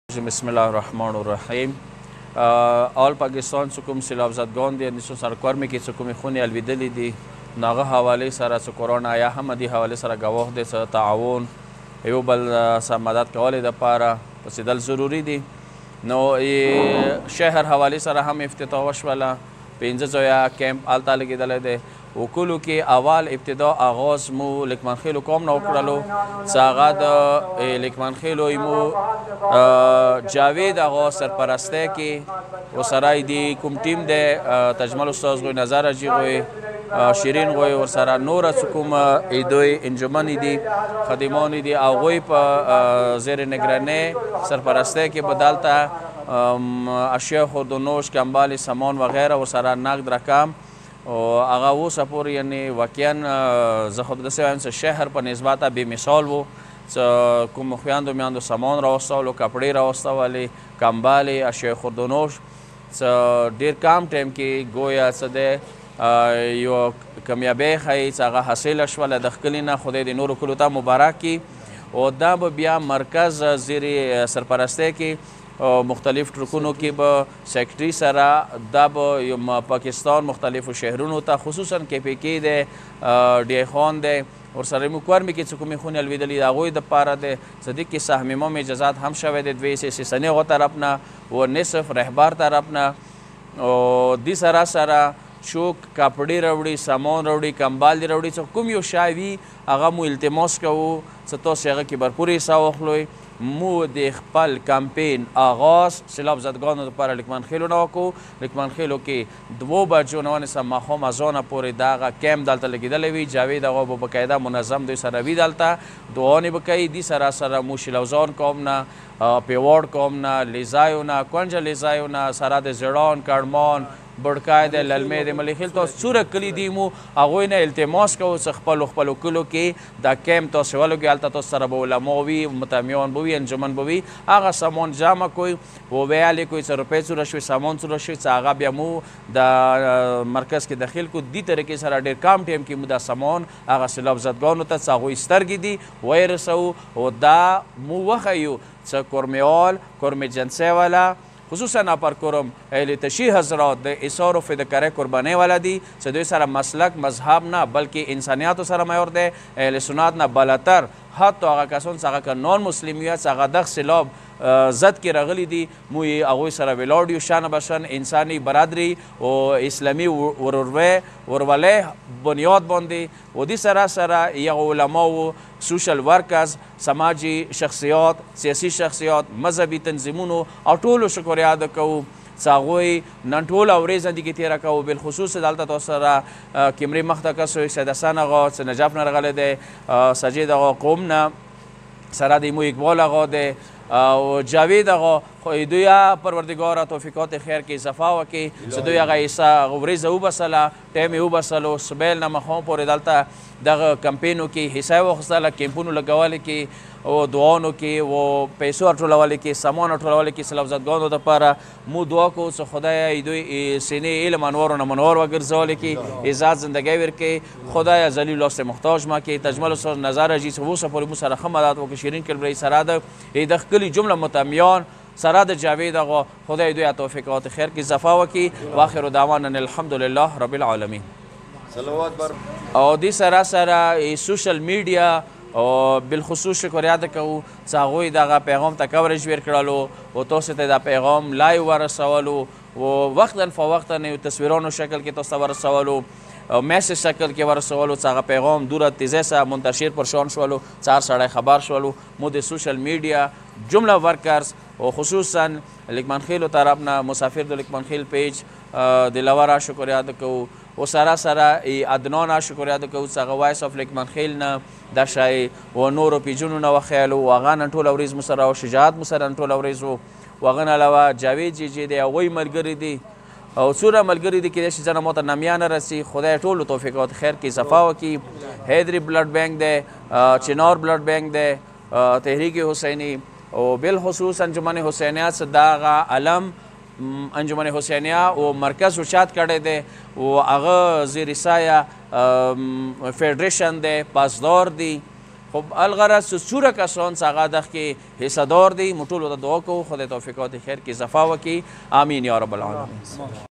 || Allama Akhlaq Hussain Shariati || Flood Relief Camp || Parachinar || Pakistan ||
پاکستان سیلاب زدگان کیلئے ۔۔
ایثا فداکاری کا مظاہرہ کرتے ہوئے آج پورے کرم ایجنسی میں سیکرٹری انجمن حسینیہ کی نگرانی میں " مالی تعاون " کا آغاز آج بروز اتوار 28/08/2022 سے ہوا ۔
اور دیہات کے حوالے سے سب  سے پہلے قوم لقمان خیل سے ایثار کے عمل کا آغاز ہوا اور بہت کم عرصے میں ایک لاکھ بیس ہزار روپے /120000 اور اس کے علاوہ کافی تعداد میں کمبل، کپڑے اور دیگر اشیائے خوردونوش جمع ہوا اور یہ کمپین ایک ہفتے تک لقمان خیل میں جاری رہیگا ۔
کمپین کی سرپرستی آغا جاوید حسین جعفری اور مرکزی انجمن حسینیہ لقمان خیل کریگا ۔